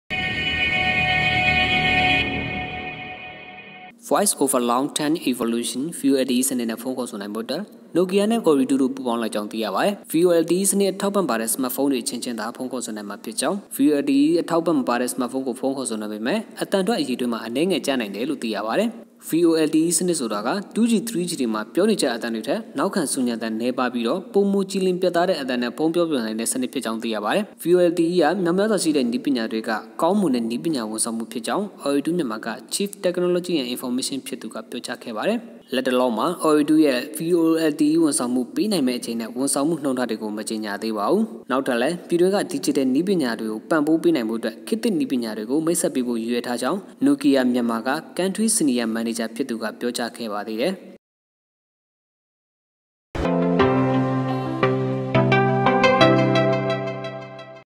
Voice Over Long Term Evolution
Voice Over Long Term Evolution (VoLTE) စနစ္နဲ႔ ဖုန္းေခၚႏိုင္ဖို႔အတြက္ Nokia နဲ႔ Ooredoo တို႔ပူးေပါင္းလိုက္ေၾကာင္းသိရပါတယ္။